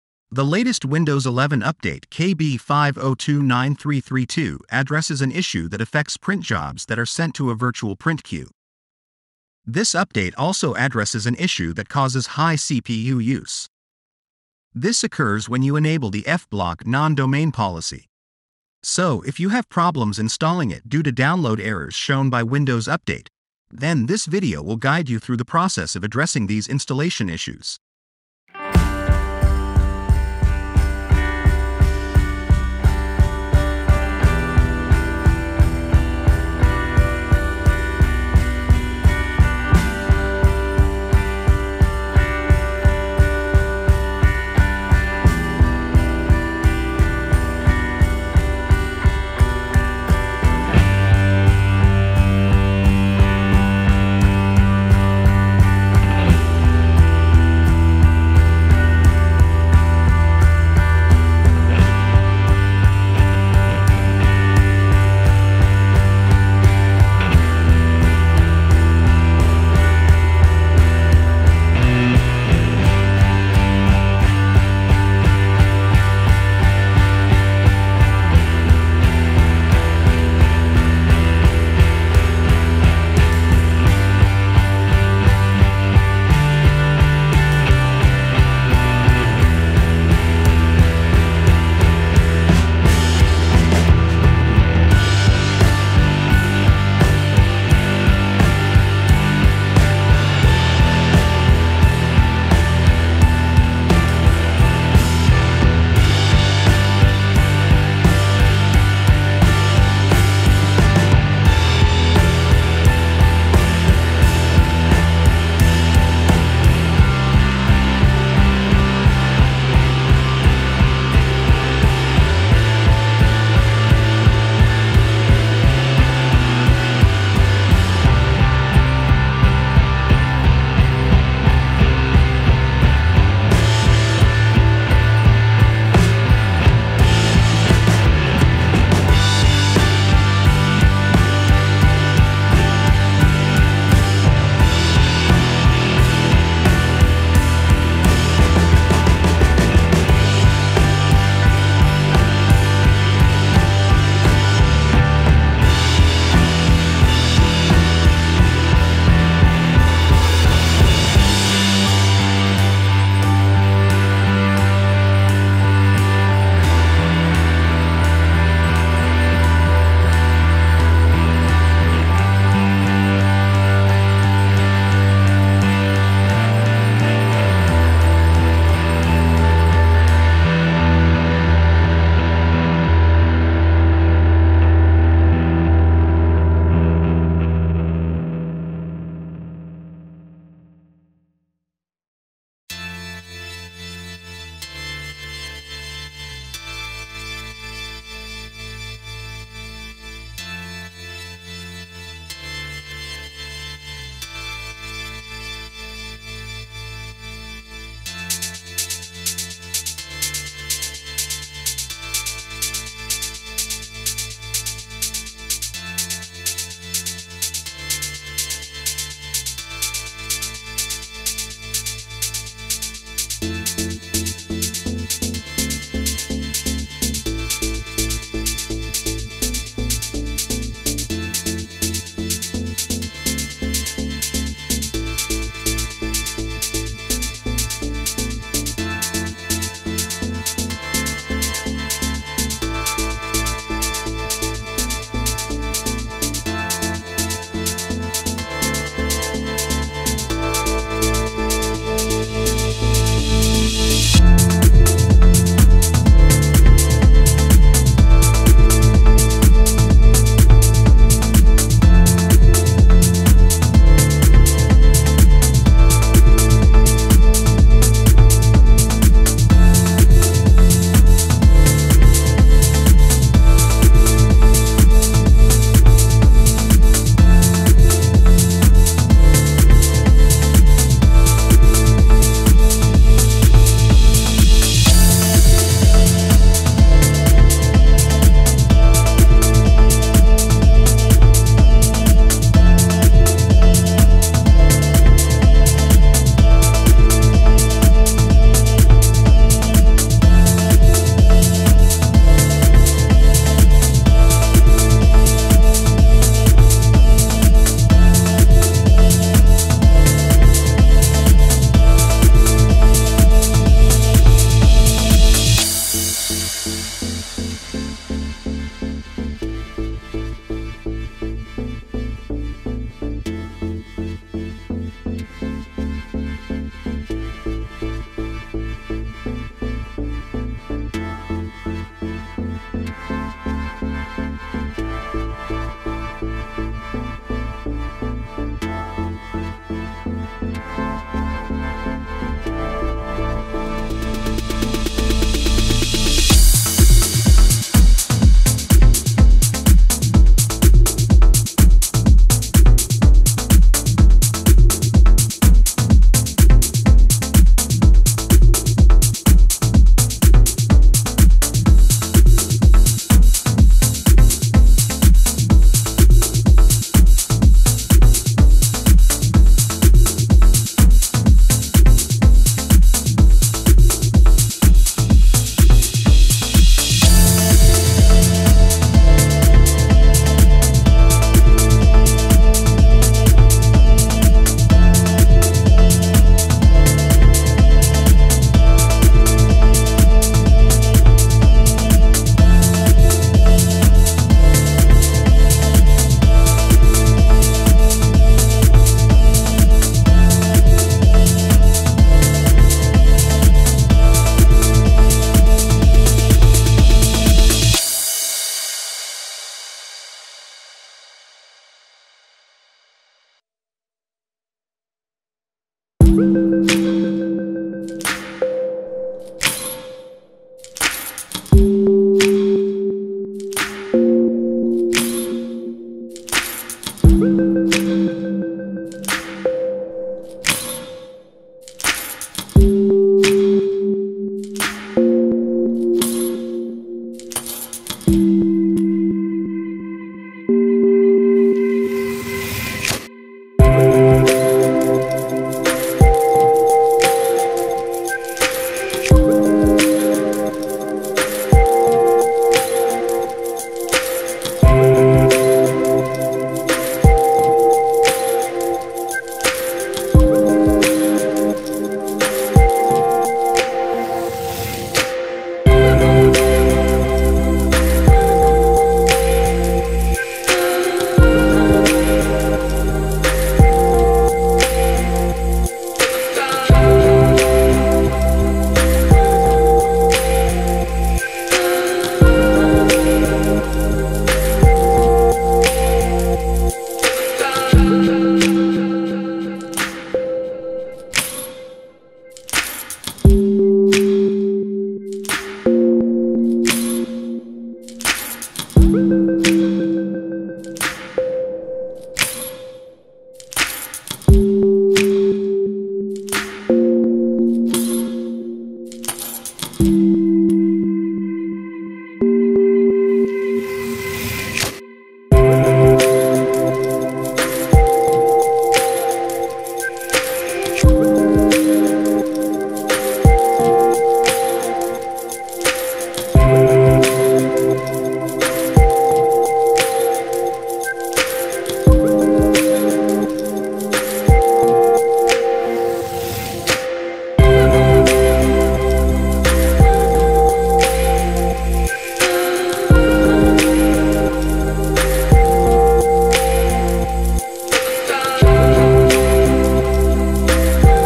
FIX Windows Update KB5029332 Not Installing or Downloading In Windows 11
The latest windows 11 update KB5029332 This update addresses an issue that affects print jobs that are sent to a virtual print queue. They fail without an error. This update also  addresses an issue that causes high CPU use. This occurs when you enable the f Block Non Domain policy. so If you have problems installing it due to download errors shown by Windows update, then this video will guide you through the process of addressing these installation issues.